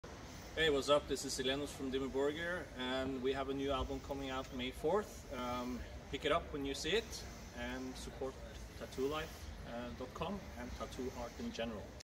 Silenoz (Dimmu Borgir)
Silenoz (Dimmu Borgir) says hello to Tattoo Life readers!